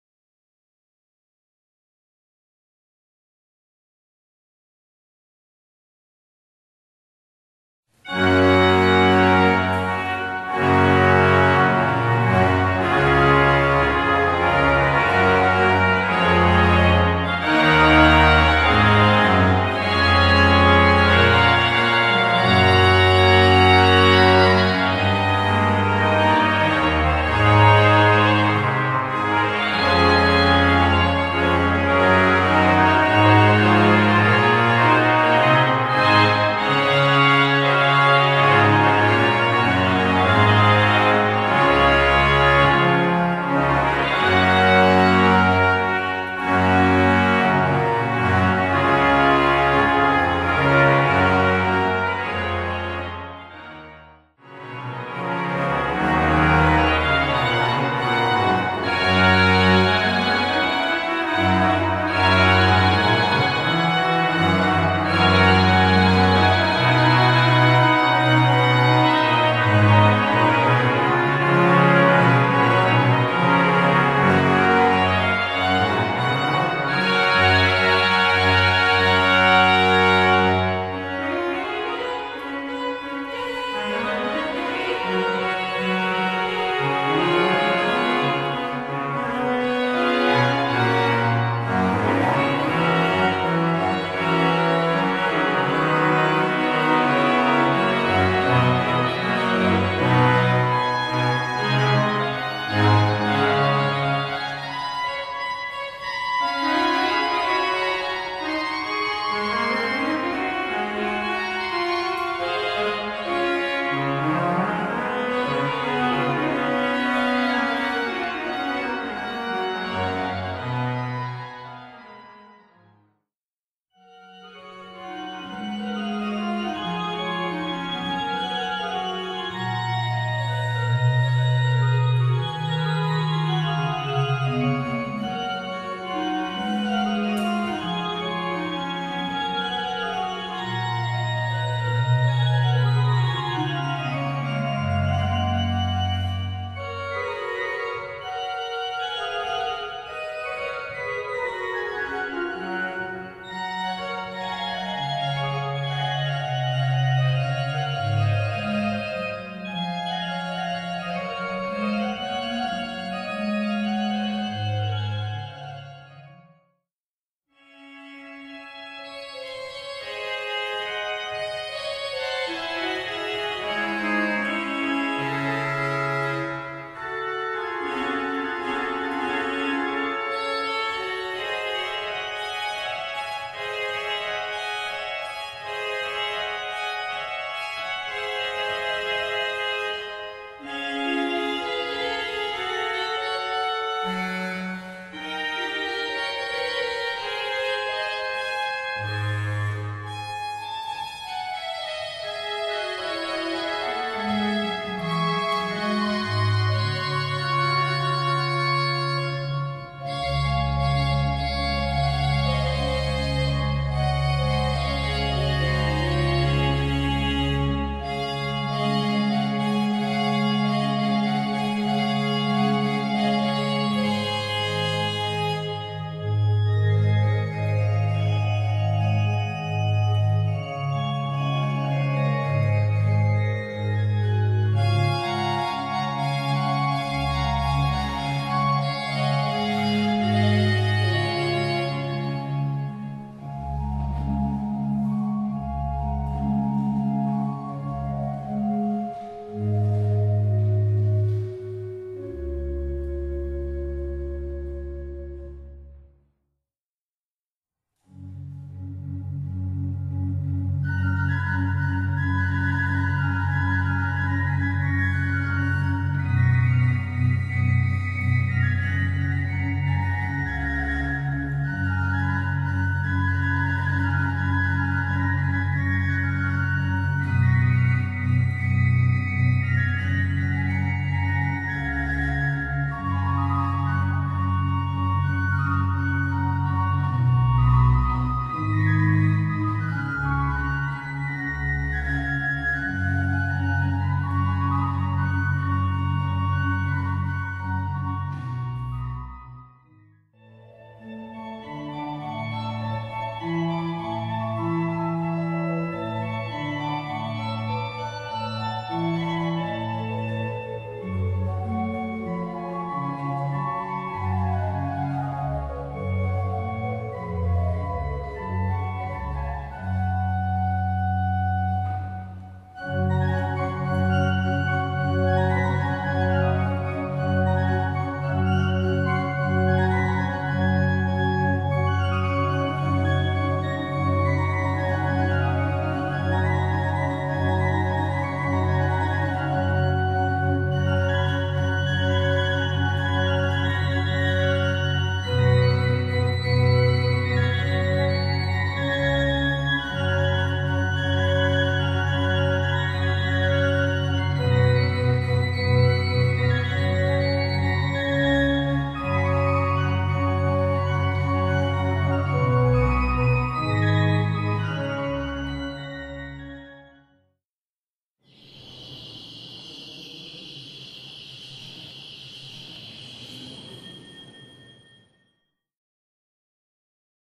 Orgue Mortroux, extraits sonores 3
Début XVIIè, l'orgue est construit de main anonyme pour l'église du Grand Séminaire de Liège où il demeure jusqu'après la Révolution, à la suite de laquelle il est racheté (1809) et déménagé une trentaine de kilomètres au cœur de la campagne de Dahlem, en l'église Sainte-Lucie de Mortroux. La Manufacture d'Orgues Thomas de Ster le restaure en 2015 en respectant scrupuleusement le large fond historique qui couvre plus de 4 siècles de facture liégeoise !

Thomas Deserranno, concert du 12 juin 2016